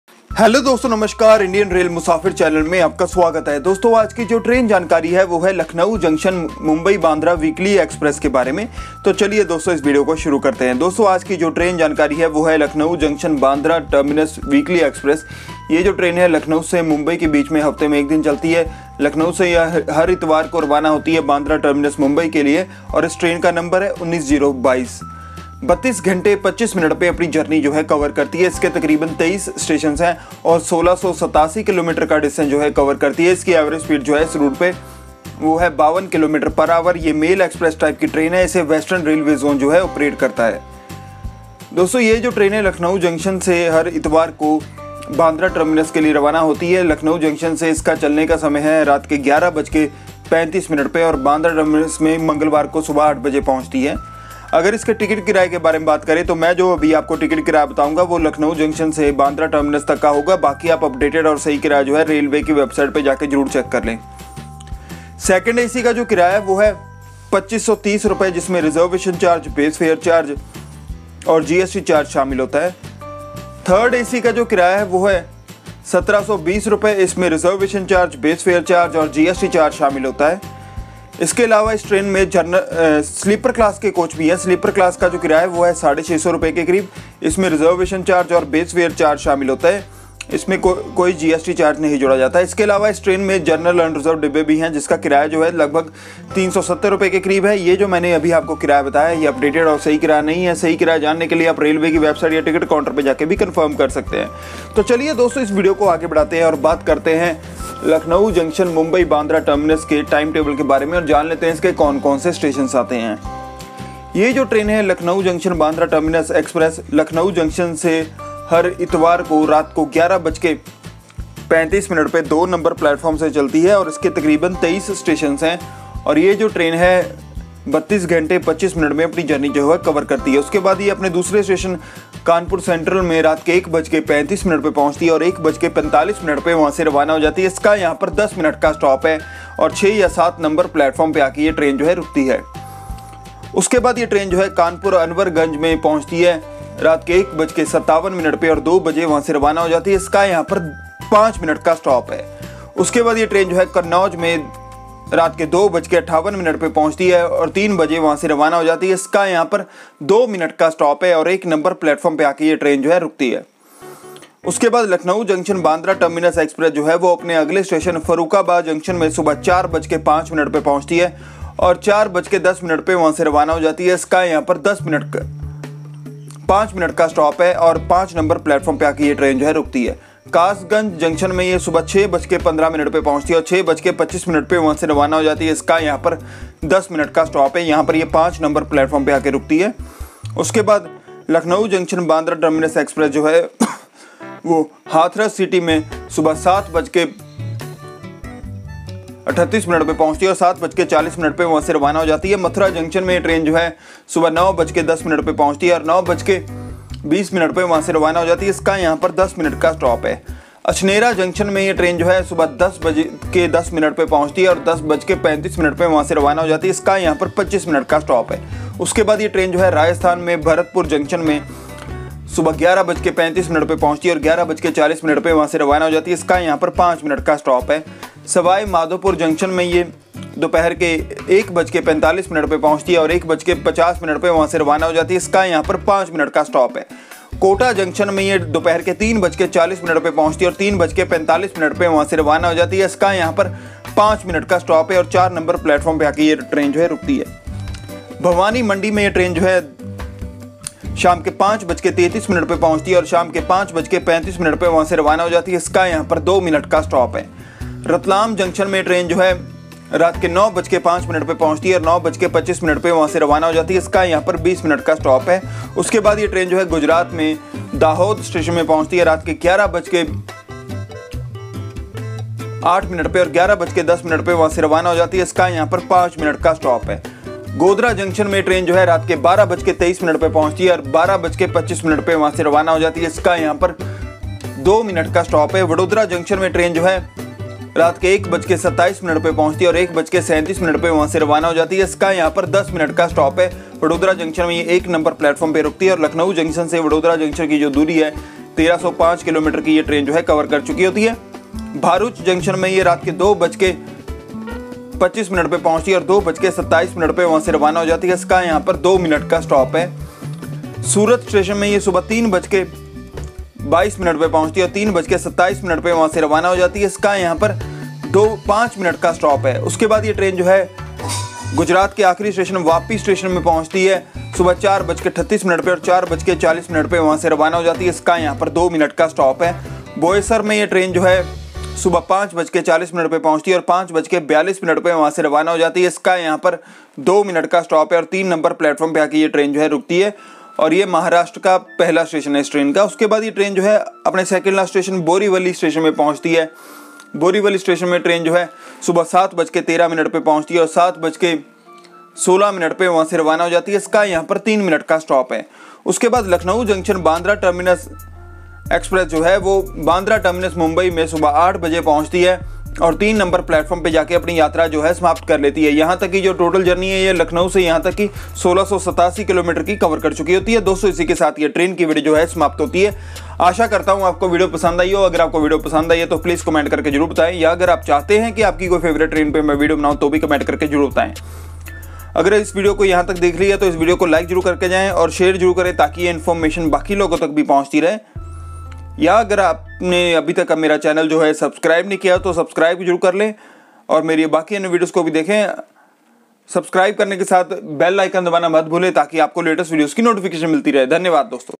Lucknow Jn. - Mumbai Bandra Express | 19022 Train | Train Information | Via kanpur, ratlam
- मुंबई बांद्रा (T.) साप्ताहिक एक्सप्रेस भारतीय रेल की प्रमुख ट्रेन में से एक है जो लखनऊ जंक्शन एनईआर और बांद्रा टर्मिनस के बीच चलती है। ट्रेन द्वारा तय की गई कुल दूरी 1687 किमी है और ट्रेन की औसत गति समय पर स्रोत स्टेशन से 52 किमी / घंटा है। यह समय पर गंतव्य स्टेशन पर पहुंचती है। 19022 / लखनऊ जंक्शन। - मुंबई बांद्रा (T.) साप्ताहिक एक्सप्रेस में 23 ठहराव हैं। ट्रेन अगले दिनों (रविवार) को चलती है और इसमें कोच होते हैं - (2A, 3A, SL)।

19021/22 बांद्रा टर्मिनस - लखनऊ साप्ताहिक एक्सप्रेस वडोदरा जंक्शन, रतलाम जंक्शन, कोटा जंक्शन, मथुरा जंक्शन, कानपुर सेंट्रल से लखनऊ जंक्शन तक जाती है और इसके विपरीत।

ट्रेन के महत्वपूर्ण पड़ाव हैं:

बांद्रा टर्मिनस
बोरीवली
बोईसर
वापी
सूरत
भरूच जंक्शन
वडोदरा जंक्शन
गोधरा जंक्शन
दाहोद
रतलाम जंक्शन
भवानी मंडी
कोटा जंक्शन
सवाई माधोपुर
भरतपुर जंक्शन
अछनेरा जंक्शन
मथुरा जंक्शन
हाथरस सिटी
कासगंज जंक्शन
फर्रुखाबाद जंक्शन
कन्नौज
कानपुर अनवरगंज
कानपुर सेंट्रल
लखनऊ जंक्शन

19022/ Lucknow Jn. - Mumbai Bandra (T.) Weekly Express is one of the prominent train of Indian railways that runs between Lucknow Junction NER and Bandra Terminus. The total distance covered by the train is 1687km and average speed of the train is 52km/hr Train departs from Source station at time and it reaches at Destination station at time. 19022/ Lucknow Jn. - Mumbai Bandra (T.) Weekly Express has 23 stoppages. Train runs on following days (Sunday) and consists of coaches – (2A, 3A, SL).

The 19021/22 Bandra Terminus - Lucknow Weekly Express runs via Vadodara Junction, Ratlam Junction, Kota Junction, Mathura Junction, Kanpur Central to Lucknow Junction and vice versa.

The important halts of the train are:

Bandra Terminus
Borivali
Boisar
Vapi
Surat
Bharuch Junction
Vadodara Junction
Godhra Junction
Dahod
Ratlam Junction
Bhawani Mandi
Kota Junction
Sawai Madhopur
Bharatpur Junction
Achhnera Junction
Mathura Junction
Hathras City
Kasganj Junction
Farrukhabad Junction
Kannauj
Kanpur Anwarganj
Kanpur Central
Lucknow Junction